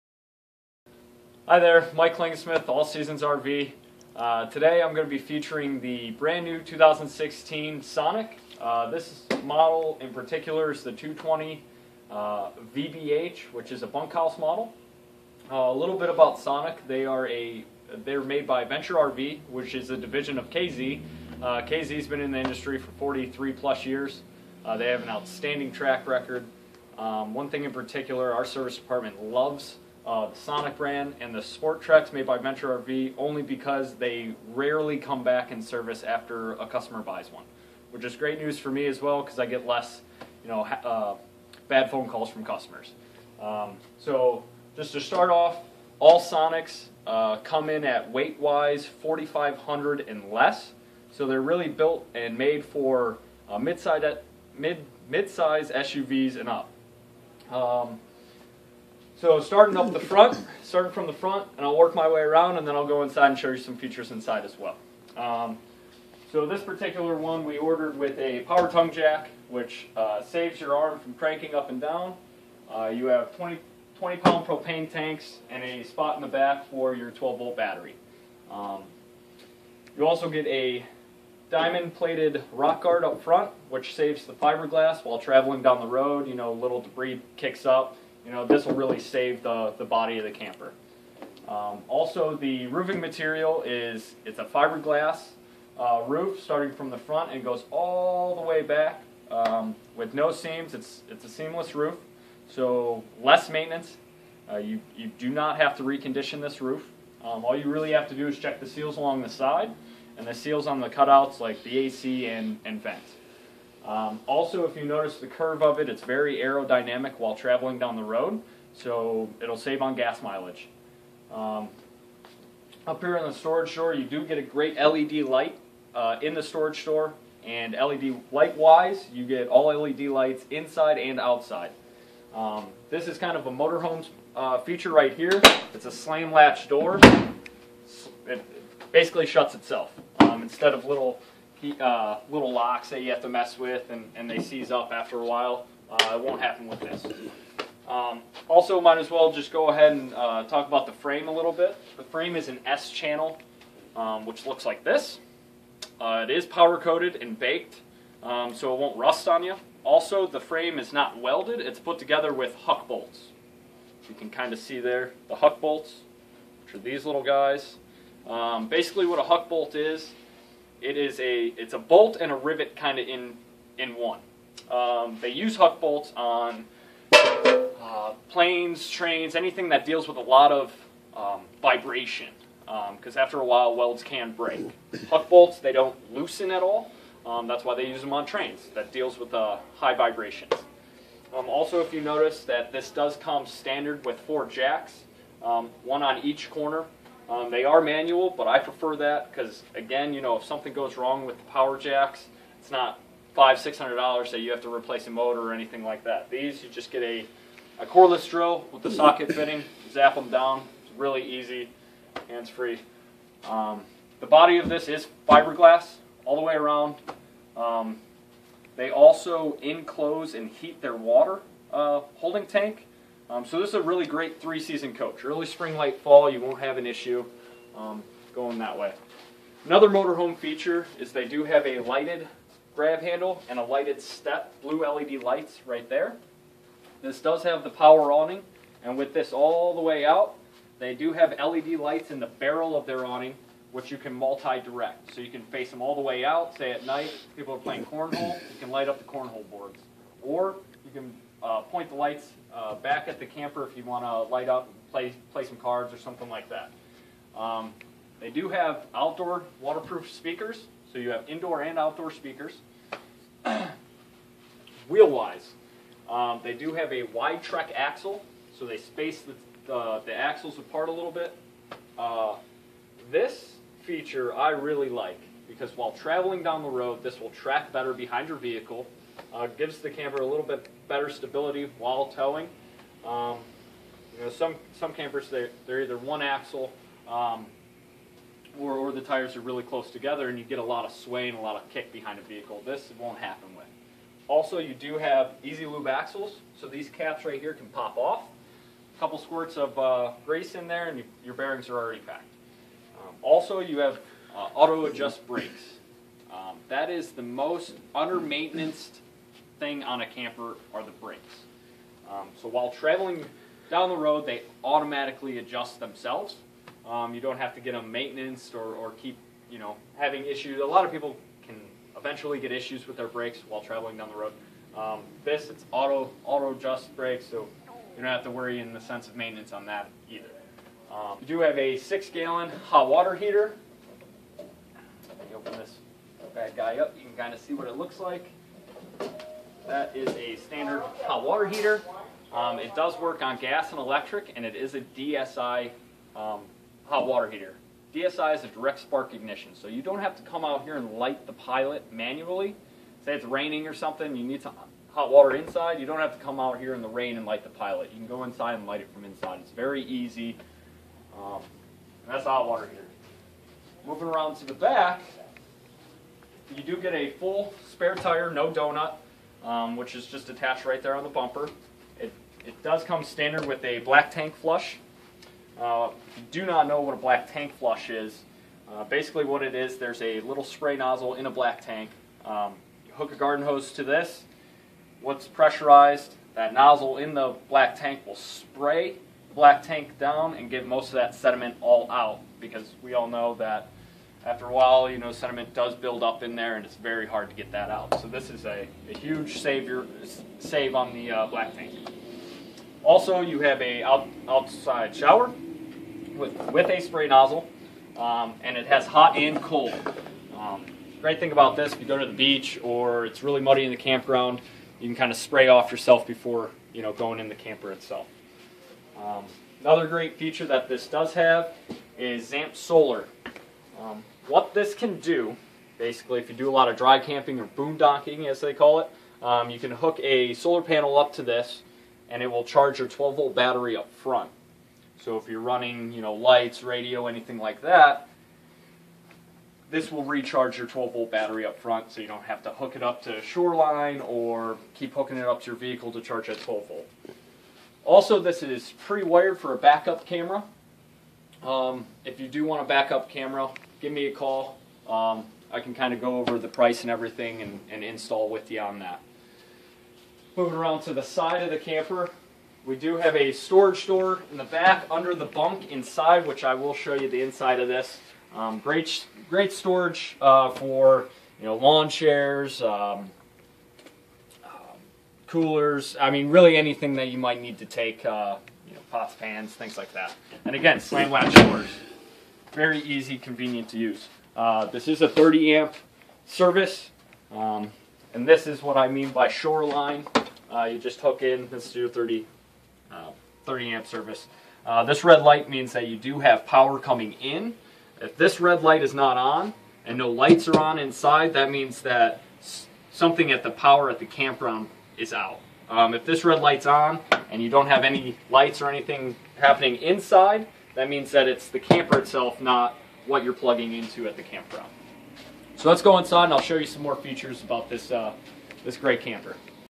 Sonic 220VBH Light Weight Camping Trailer with Bunks at All Seasons RV
Check out the exterior of the 2016 Sonic 220VBH. The Sonic 220VB is a light weight camping trailer with bunks - weighs only 4390 lbs and can sleep up to 6. Mike Klingensmith - All Seasons RV's salesman explains the Sonic 220VBH's exterior is quite impressive for a small camping trailer and includes many features you would find on a much larger and more expensive travel trailer or motorhome.